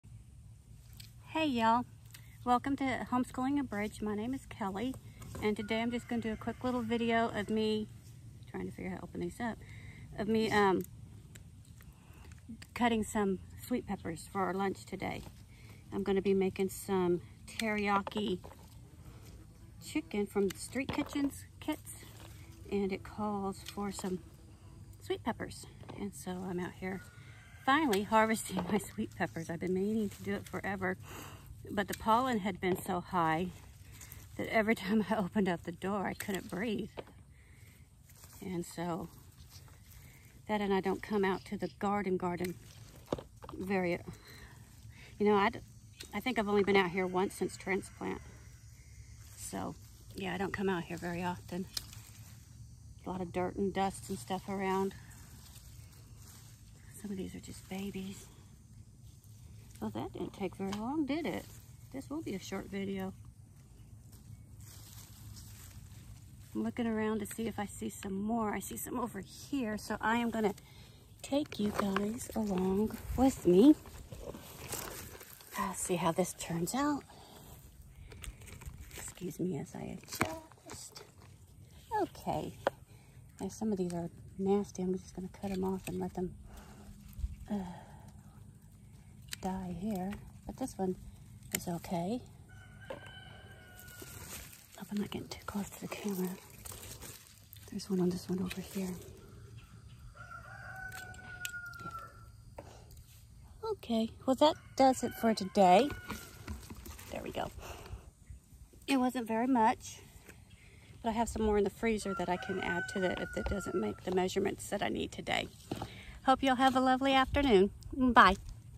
A Quick Sweet Banana Pepper Harvest
Just a quick take along as I harvest peppers for our lunch.